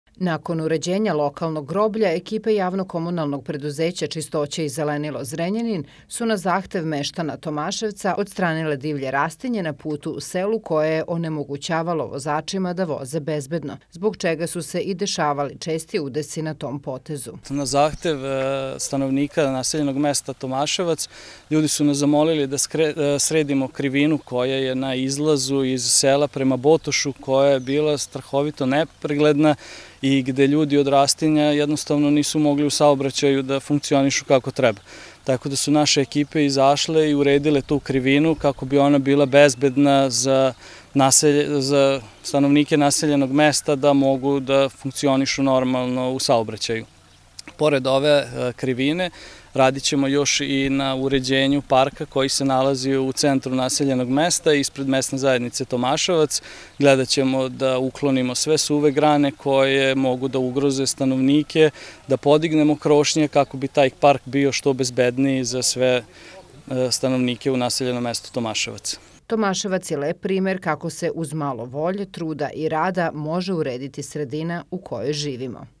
ČISTOĆA UREĐUJE PARK U TOMAŠEVCU
JKP Čistoća i zelenilo je nakon uredjenja lokalnog groblja u Tomaševcu, sredila i divlje rastinje na putu koje je otežavalo lokalni saobraćaj.U planu je i sredjivanje parka.